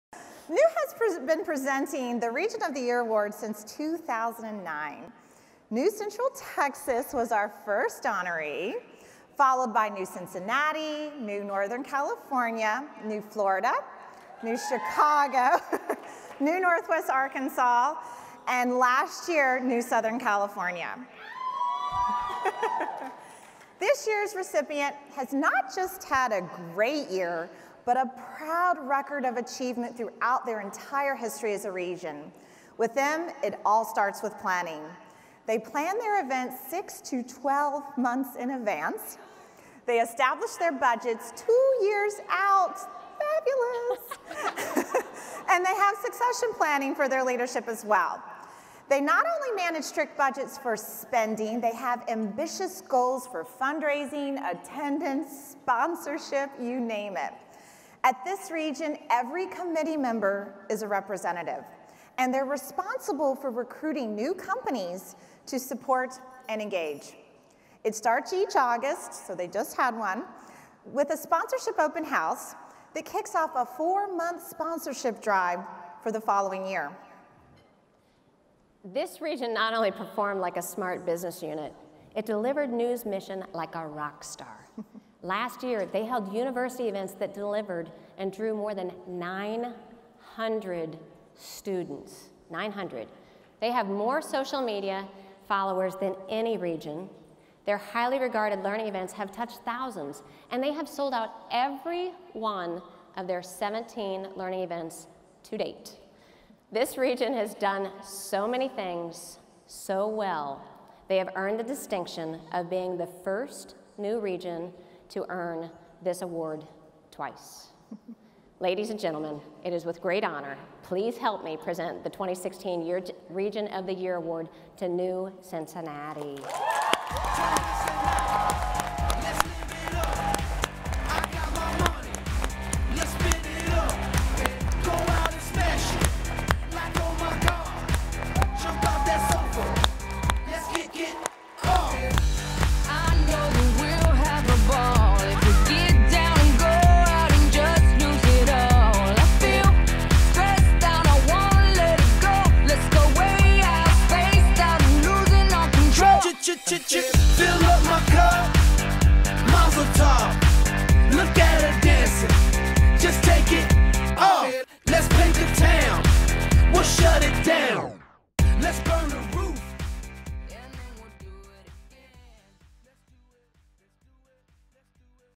Region of the Year Award at the NEW Leadership Summit 2016
NEW Cincinnati is awarded the NEW's Region of the Year.